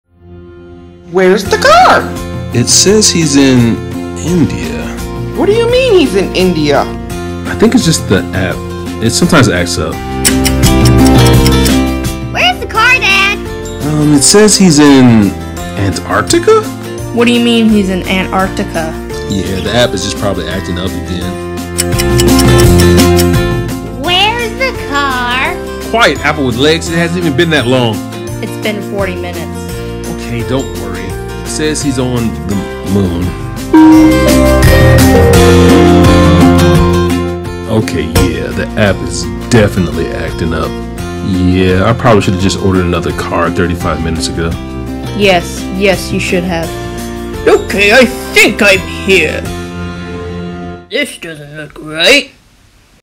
Animated Short - The Driver
This is what happens whenever you order a ride and its late...I'm not 100% positive of this.

Remember to have an amazing day!  /ᐠ｡ꞈ｡ᐟ\  🐾